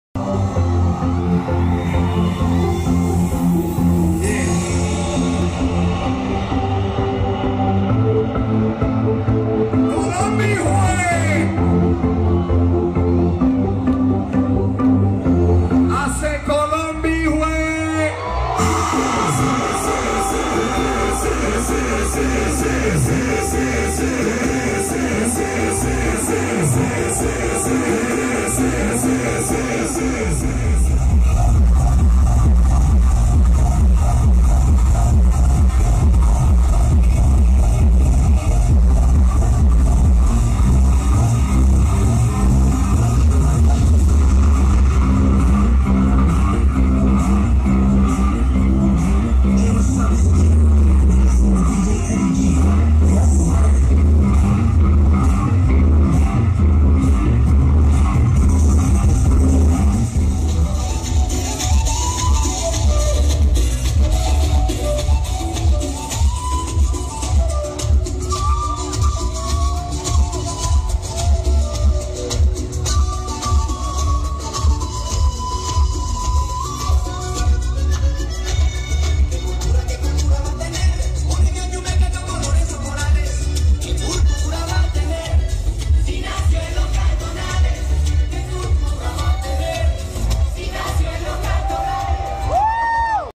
Día de la independencia de Colombia en New Jersey, USA - 2013
Celebración de el día de la independecia de Colombia (20 de Julio) en New Jersey, USA. Fue cerca a la estación Elizabeth. Lástimosamente no tomé más video.

Celebration of the Independence of Colombia (July 20th) in New Jersey, USA. Near the Elizabeth Station.